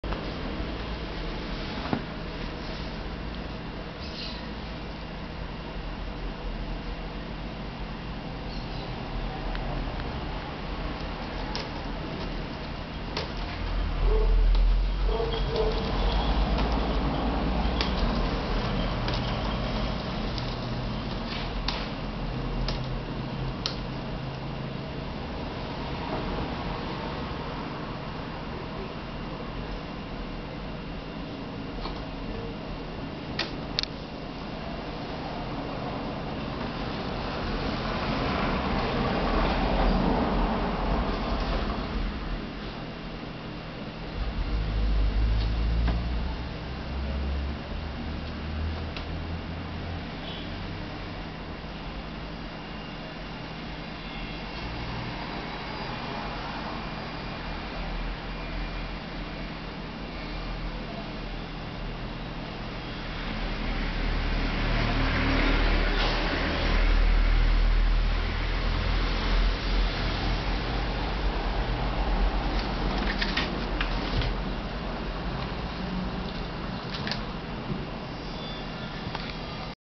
Yuna swatting at Coco
Yuna is a chicken, so she strikes from the safety of the cat tree, where Coco can't yet reach.